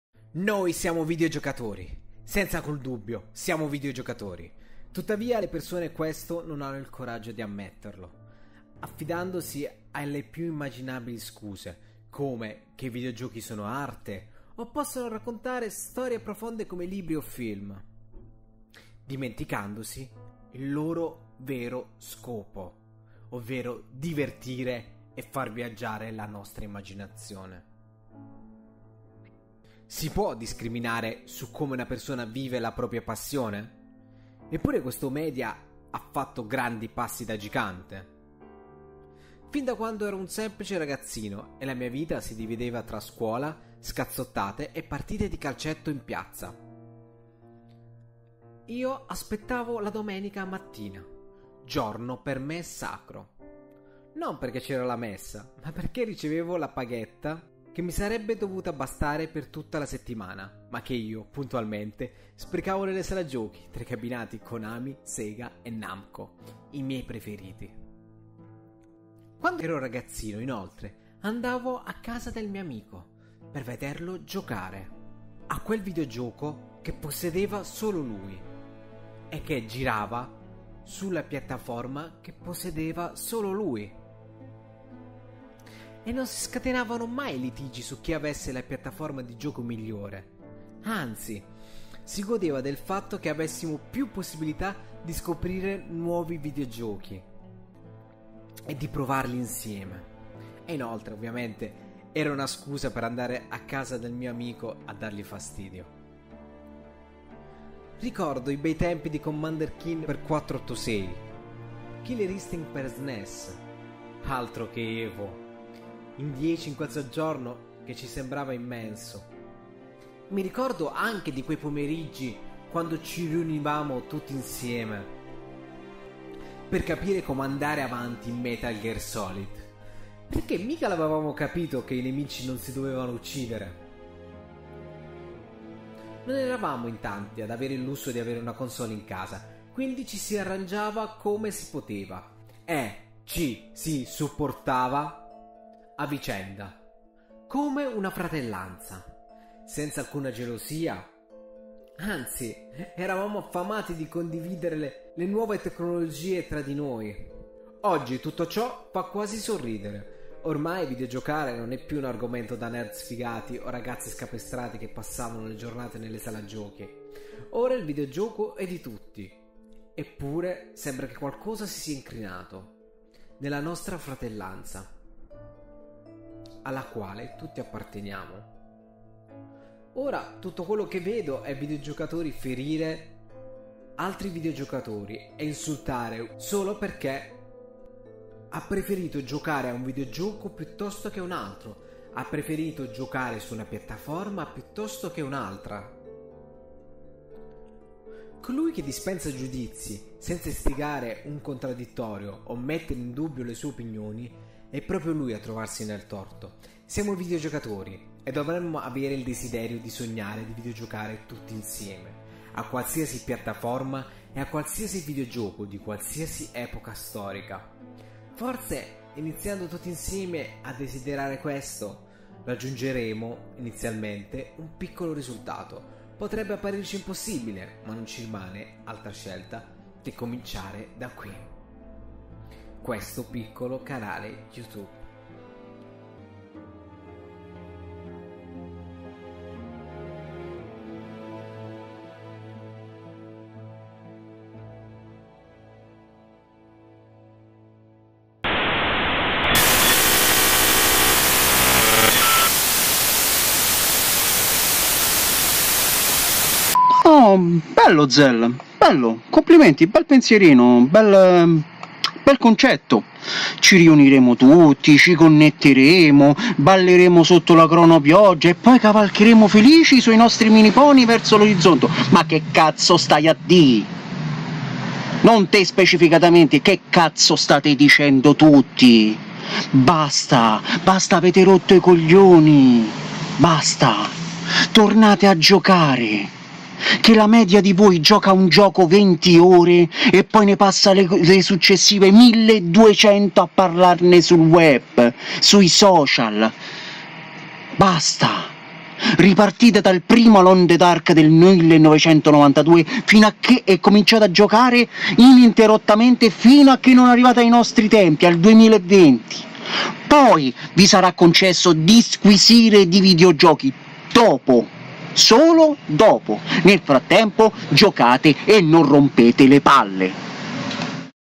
NOI VIDEOGIOCATORI DOVREMMO AMARCI, MA TUTTAVIA NON CI AMIAMO
In Questo Video tratto dell'argomento tossicità nell'ambiente videoludico e riflettendo come Boomer, quanto sia peggiorata tale tossicità, per fortuna il videogioco è rimasto intatto è la sua anima continua a vivere attraverso gli indie e grandi publisher come Devolver Digital, però l'amore fraterna quasi religioso che c'era tra noi videogiocatori è morto, ho voluto fare un punto della situazione prima della rinasciati parafrasando il monologo finale del film  Kyashan La Rinascita.

Il mio canale è solo puro sfogo e confronto

Videogiochi Brevi, giochi che si finiscono in una giornata